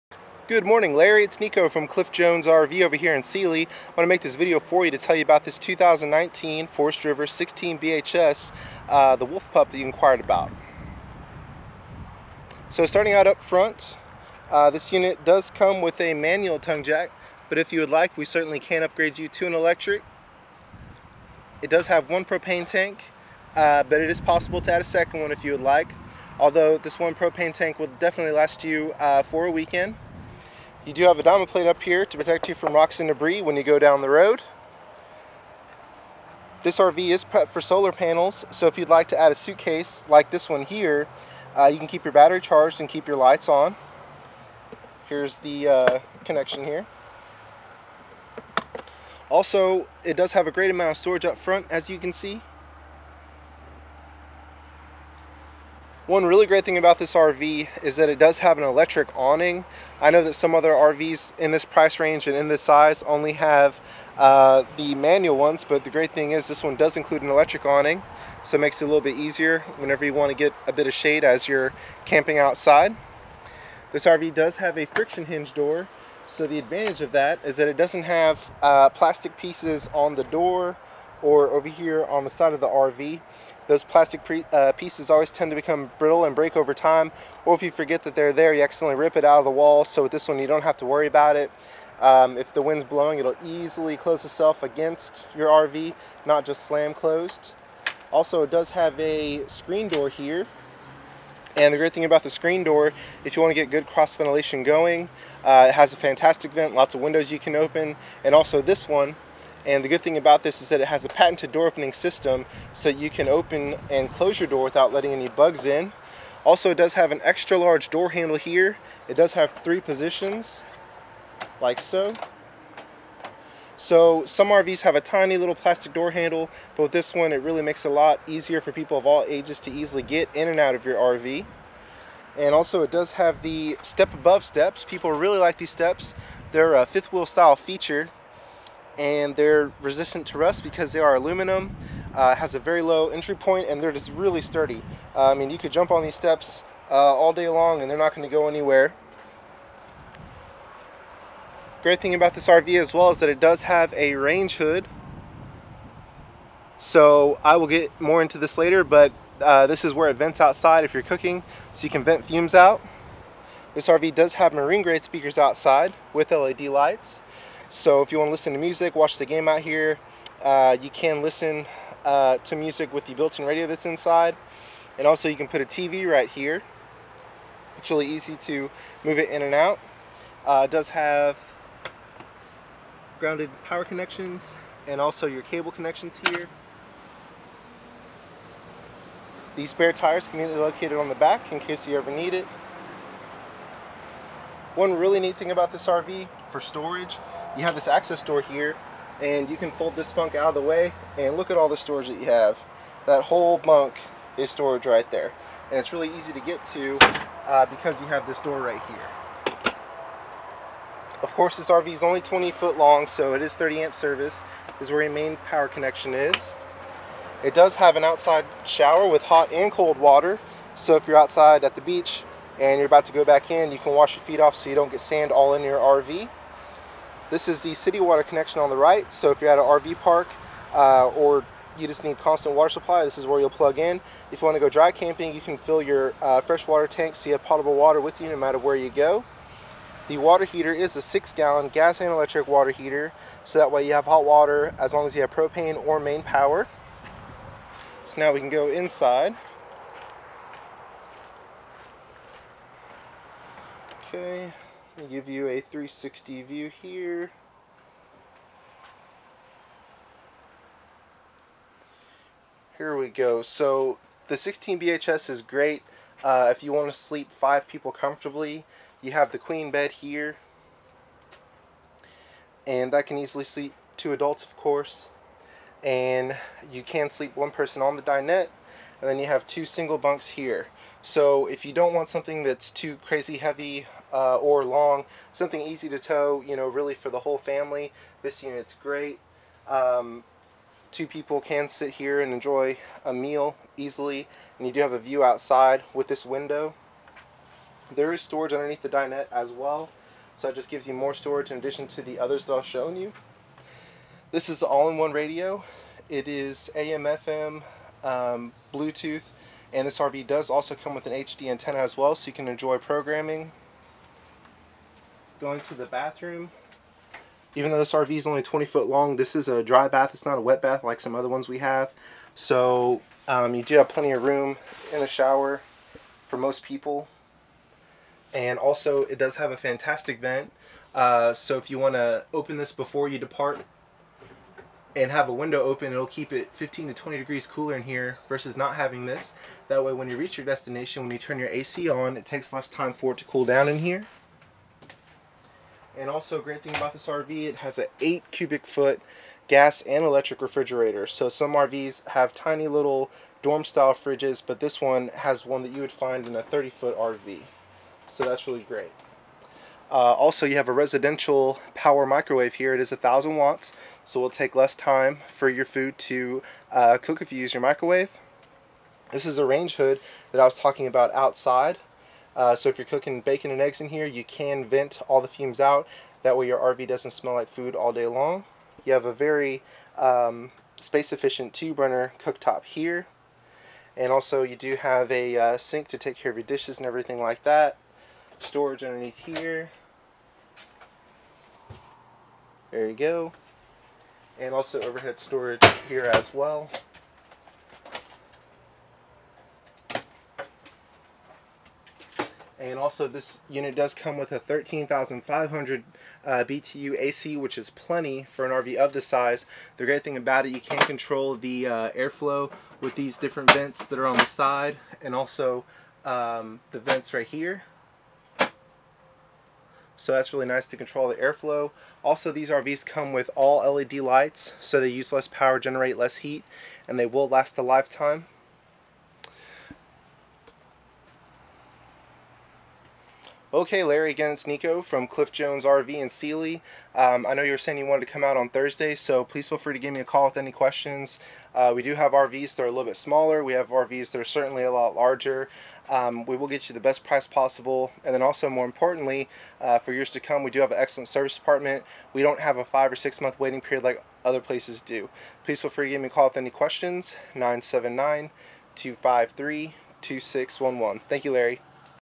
2019 Forest River Wolf Pup 16BHS
Niko shows a customer all about the Wolf Pup 16BHS!